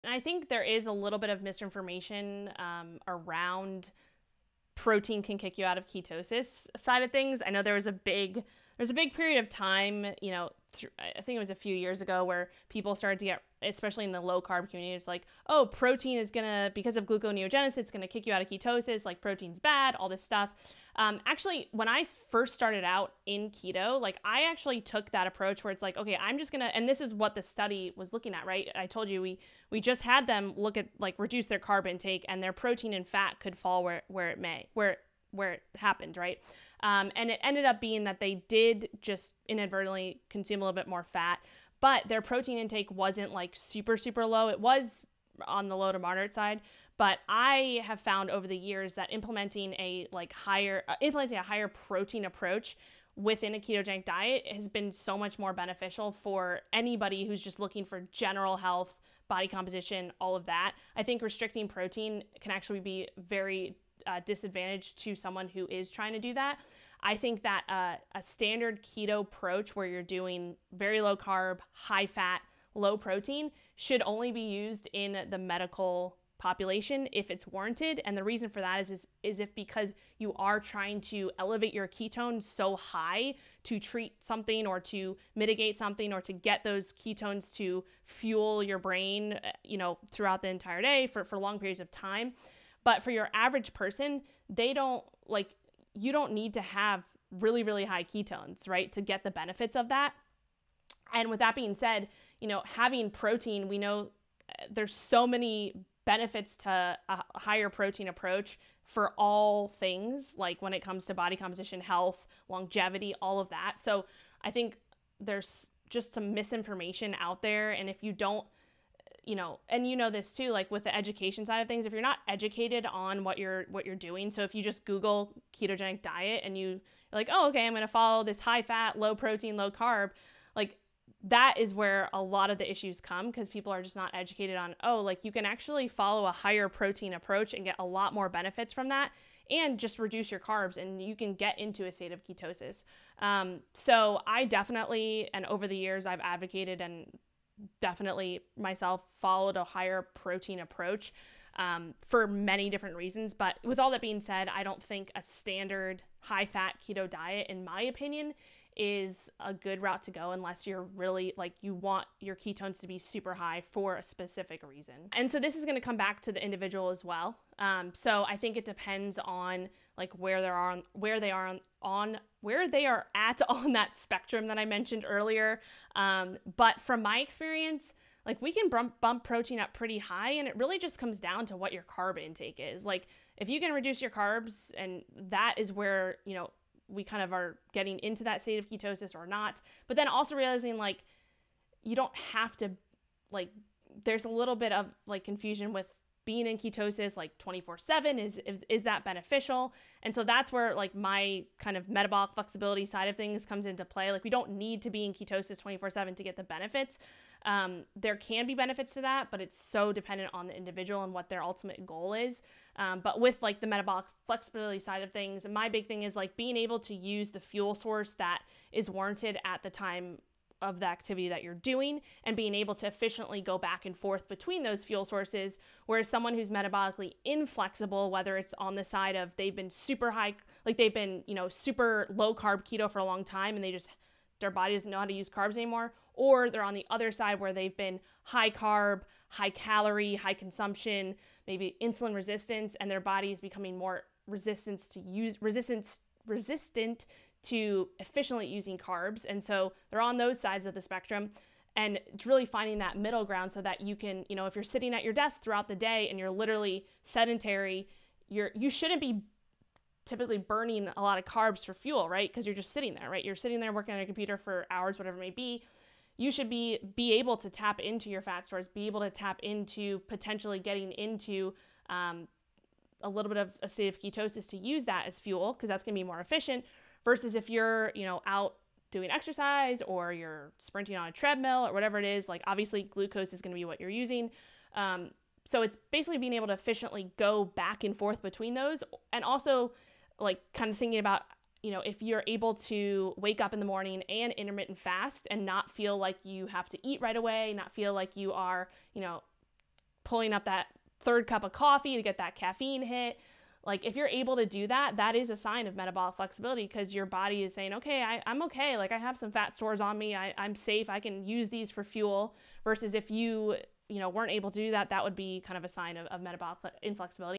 The Benefits of Becoming Metabolically Flexible
If you've been on a long-term keto diet, it's possible that you're suffering from metabolic inflexibility. There's a way to find balance between being able to tap into ketones for fuel and being able to utilize carbs for fuel when it's needed.